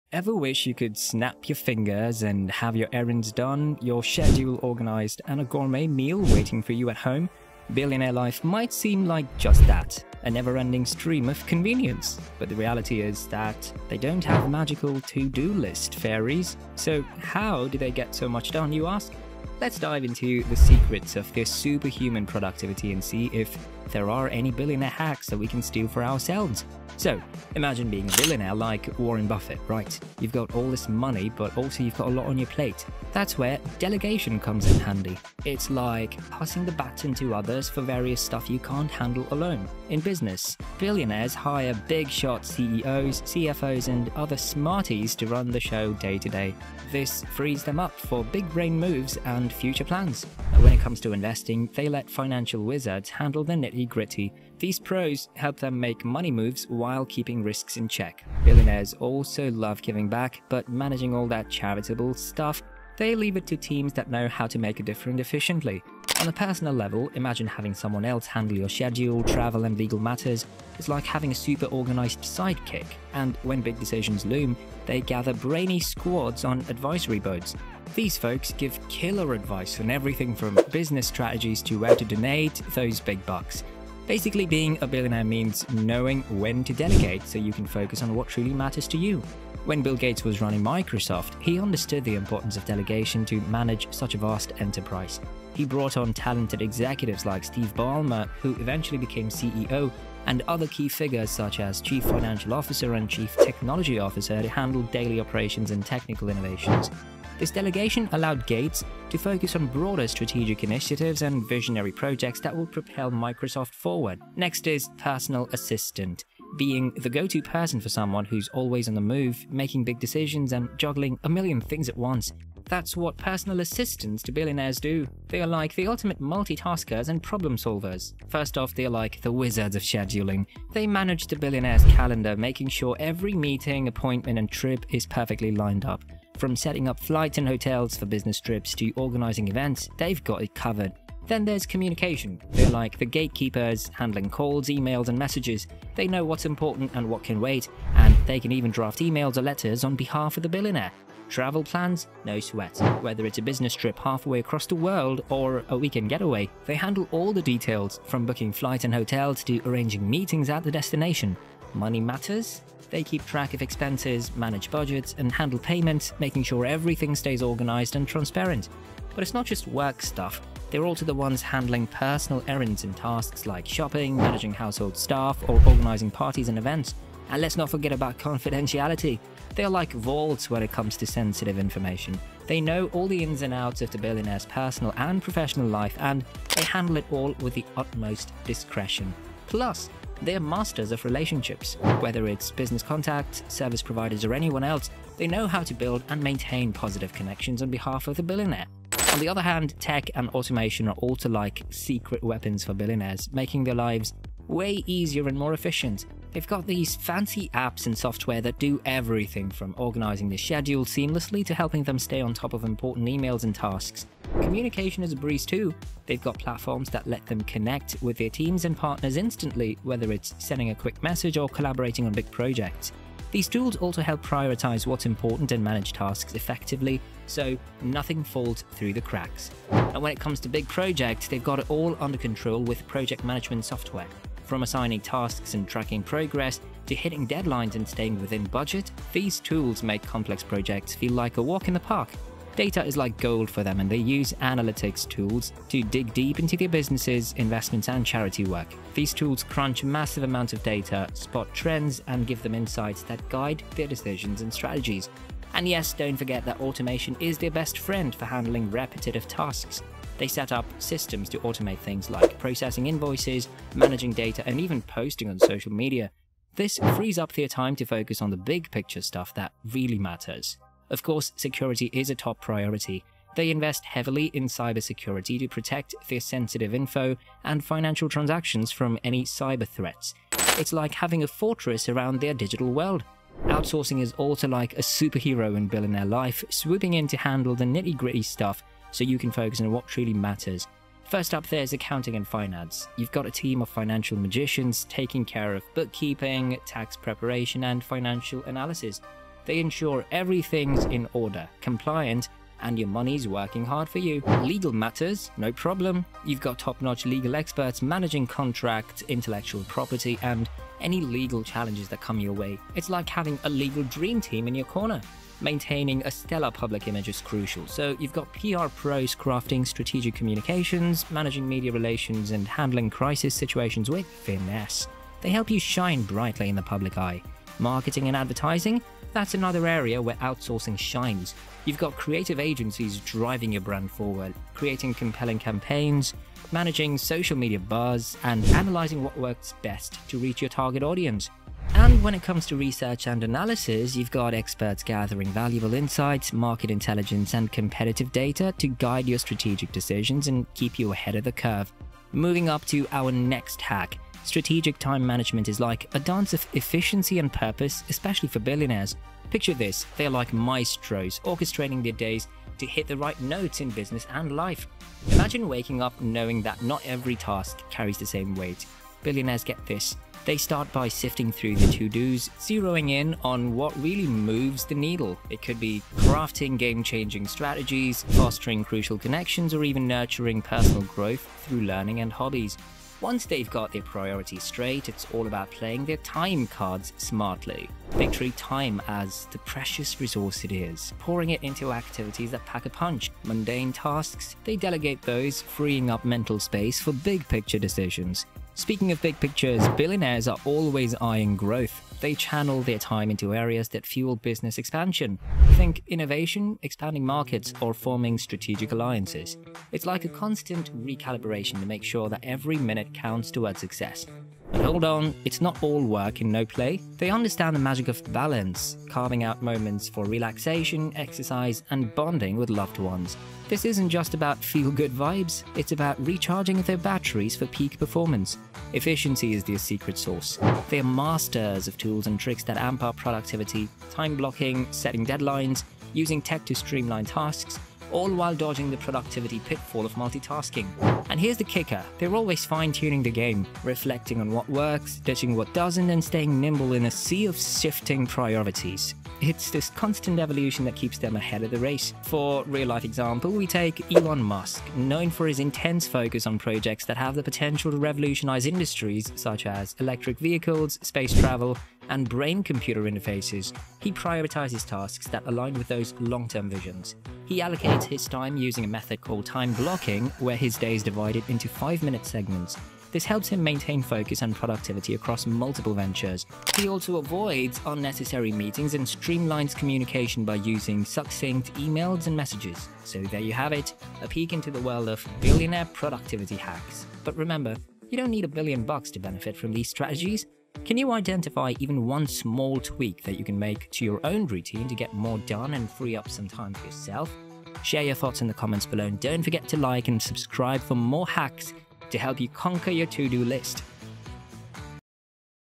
How Billionaires Get Everything Done for them
Ever wonder how billionaires get everything done effortlessly? In this video, we dive into the secrets behind their productivity, from delegation and personal assistants to tech hacks and strategic time management. Learn how they manage their schedules, finances, and projects, and discover tips you can apply to your own life. Don't miss out!

0:00 Intro
1:55 Personal Assistant
4:25 Security
4:53 Accounting and Finance
8:20 Outro